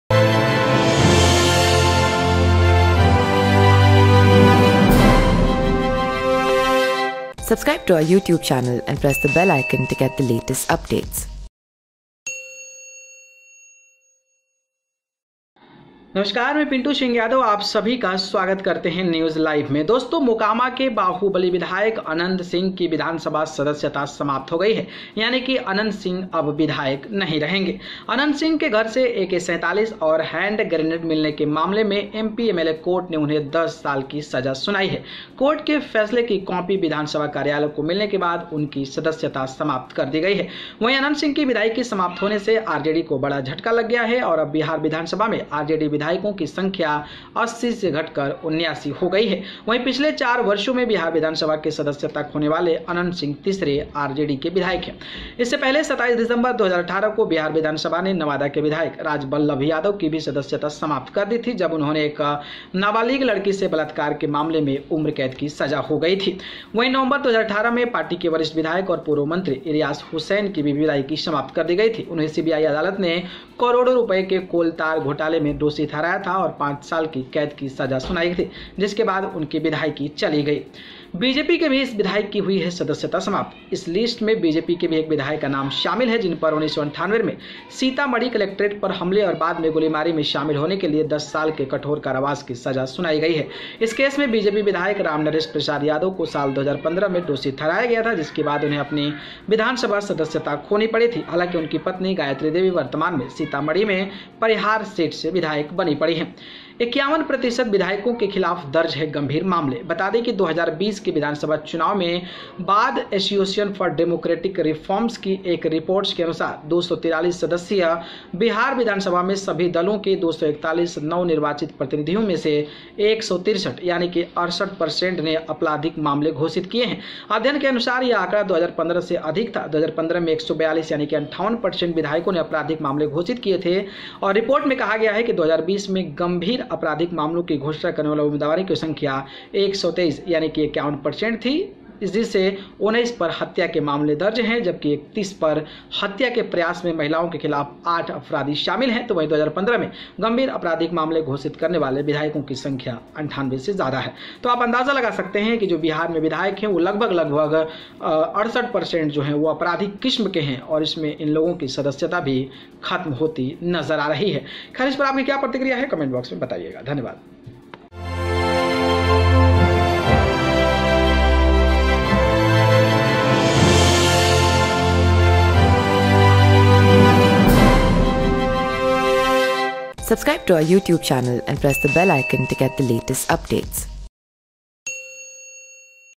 4 साल में सदस्यता खोने वाले RJD के तीसरे विधायक बने Anant Singh, जानें इस लिस्ट में कौन है शामिल....
4 साल में सदस्यता खोने वाले RJD के तीसरे विधायक बने Anant Singh, जानें इस लिस्ट में कौन है शामिल.